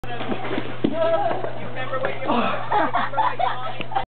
Down Donnie
Donnie Boyd's attempt at something on a tire swing July 4, 2009 at Kessler's house